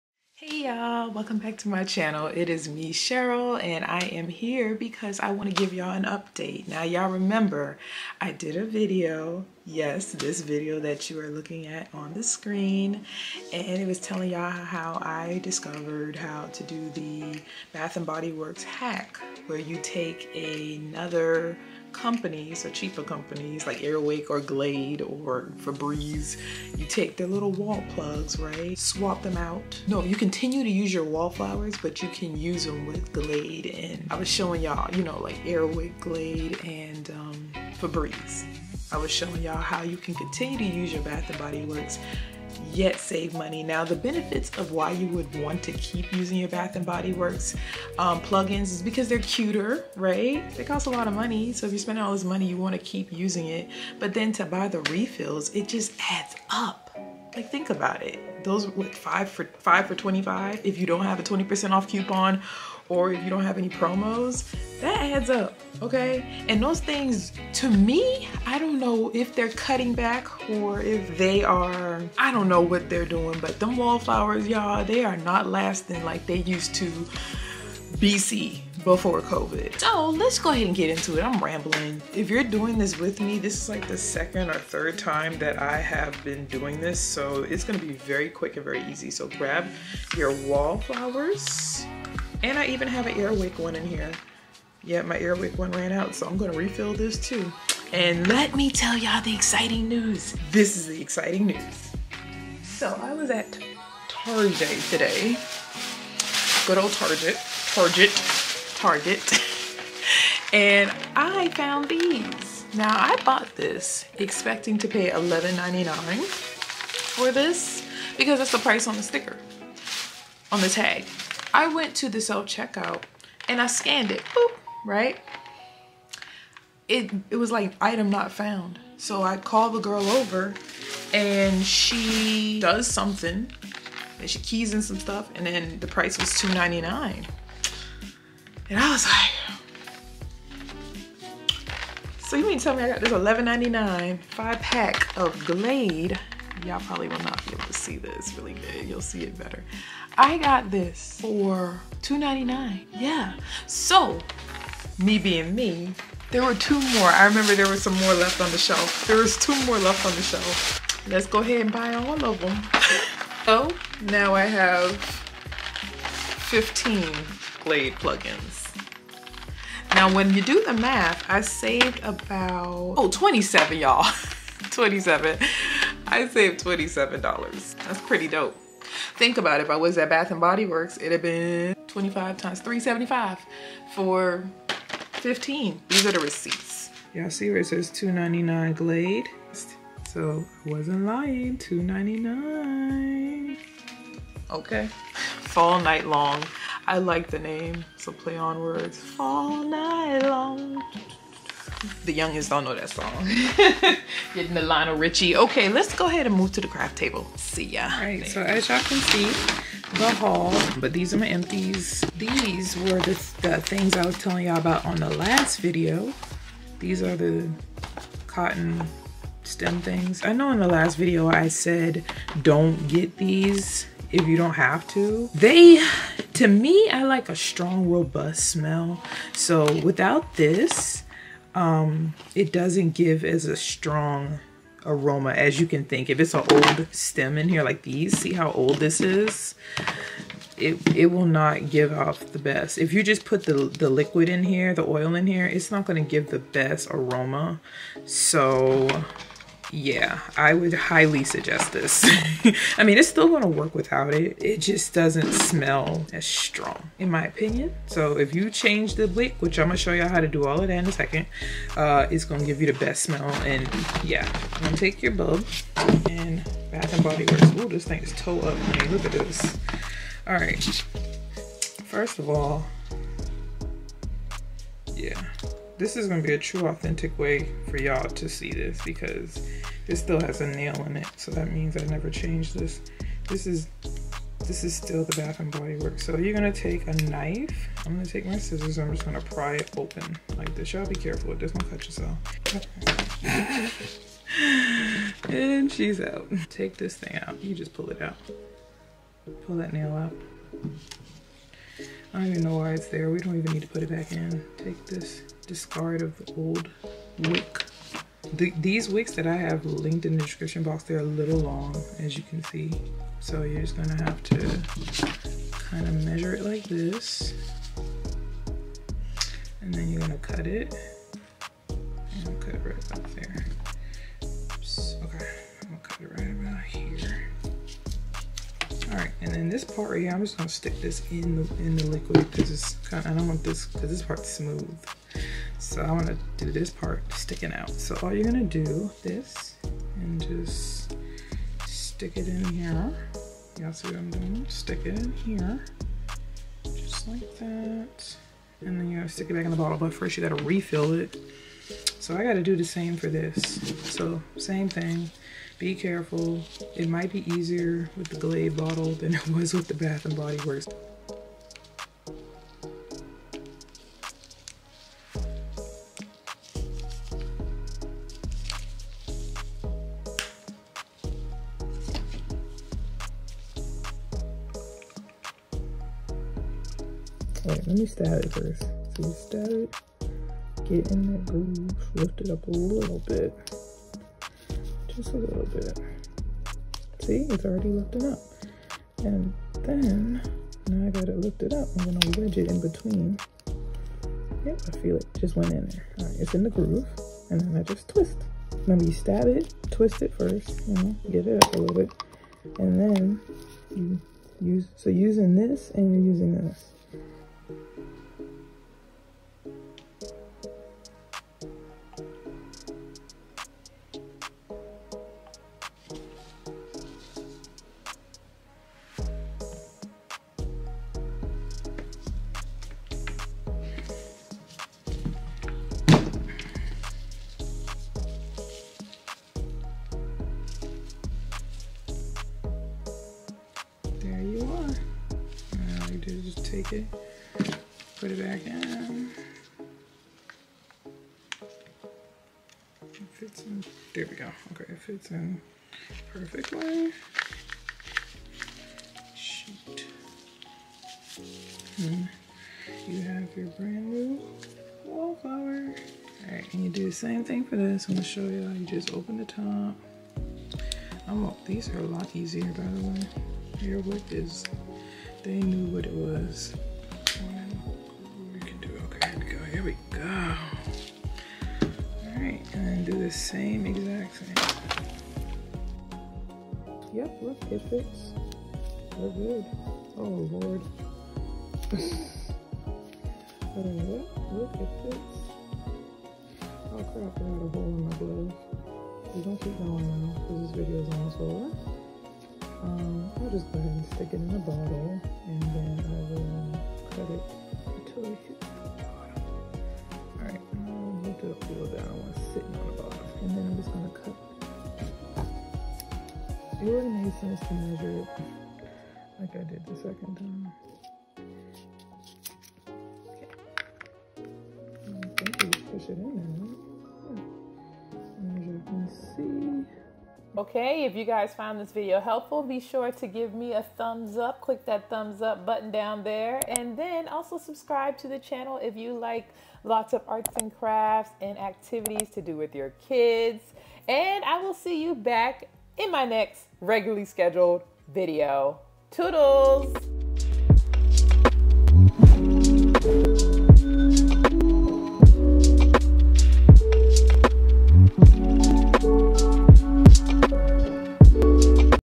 UPDATE: Refilling B&BW Wallflowers with any scent| SAVE MONEY | Activity Author
1 Month ago, I tried a Bath & Body Work Hack, and it went well but I did it again and I found out some new TRICKS! If you trying to save hundreds of dollars, watch this video!

Here is what you need:
1. Your old Wall Flower plug
2. Wall Flower refill bud
3. Air wick/Glade refill (or any other brand or room oil)
4. Cotton Diffuser refill-Here is what I ordered (You will have to cut 0.5 inches off)
5. Optional Tools you might need (Needle Nose Pliers & Flat Head screwdriver to open refill bottles)